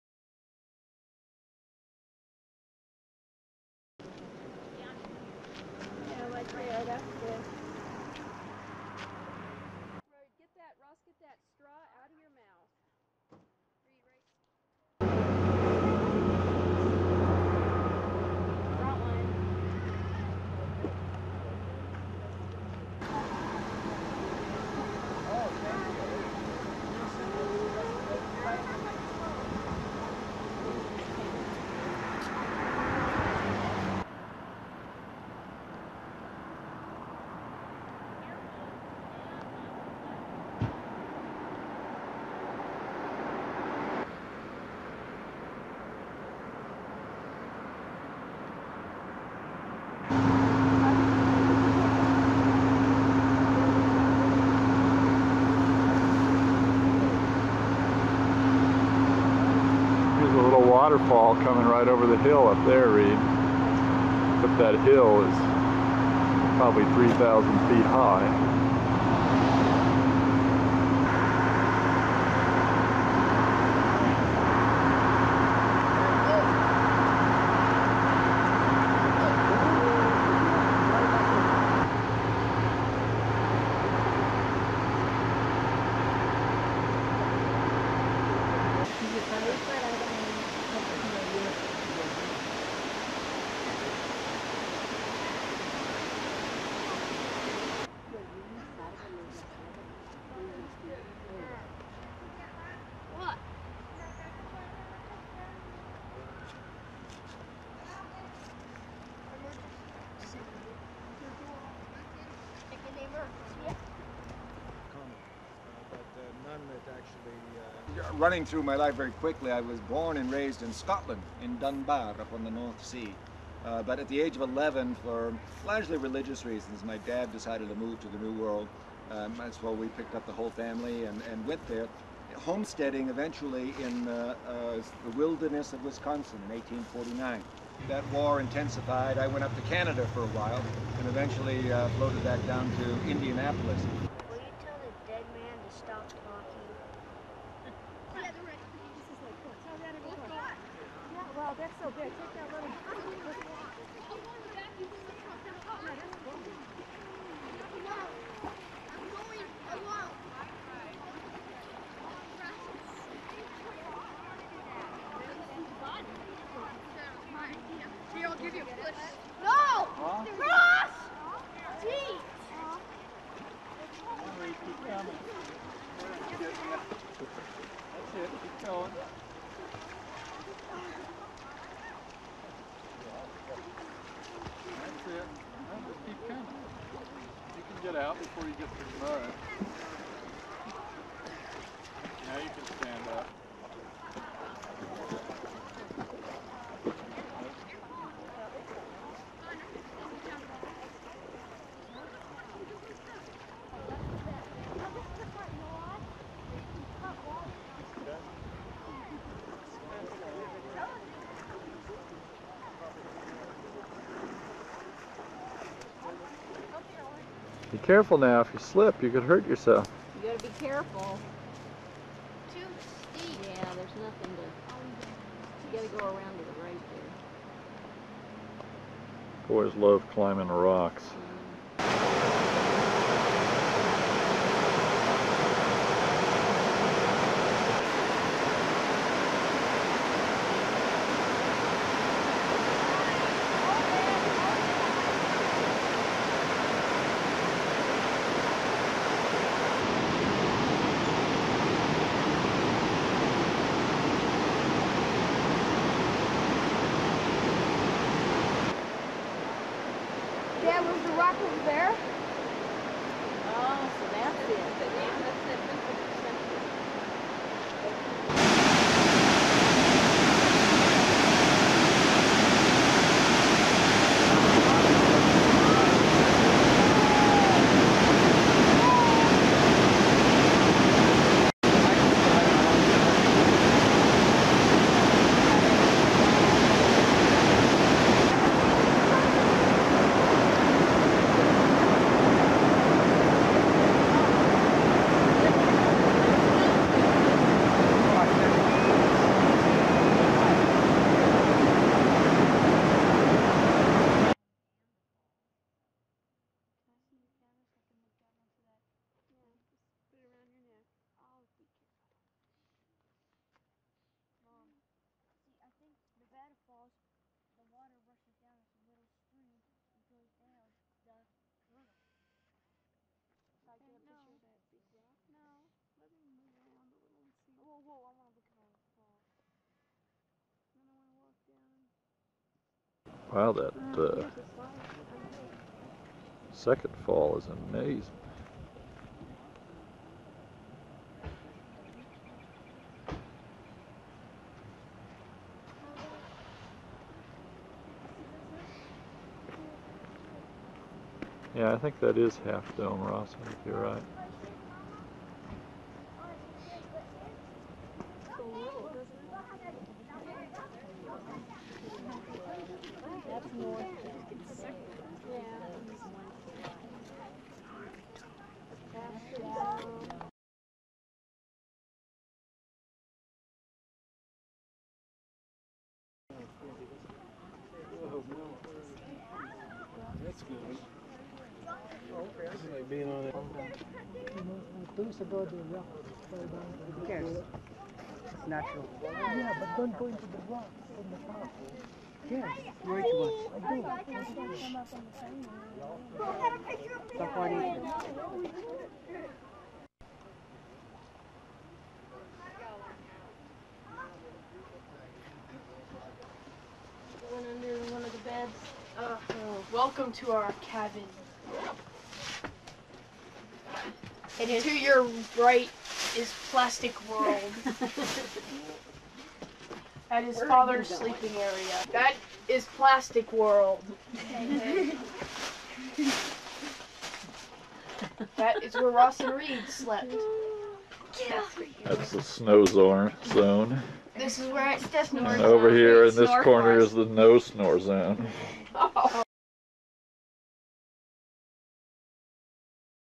Yosemite 1996
Yosemite National Park: Half Dome, El Capitan, Merced River, Bridalveil Fall, Yosemite Falls, Vernal Falls, Nevada Fall, Glacier Point. -- Yosemite National Park, Calif. (1996)